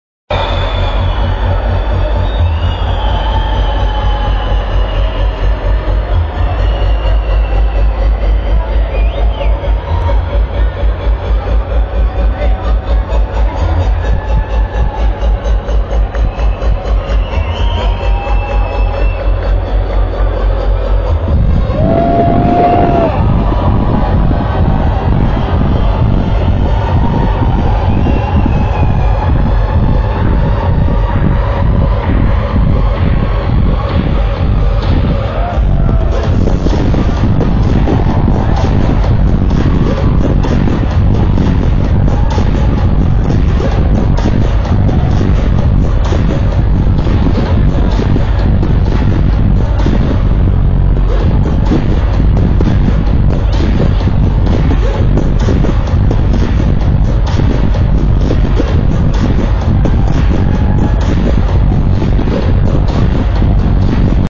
Moonbootica Hamburg, 08.06.2008
Moonbootica Party in Hamburg Hammerbrook, Germany.

Also captured with my cellphone so it's dark and has crappy sound - who cares, the party kicked ass ;)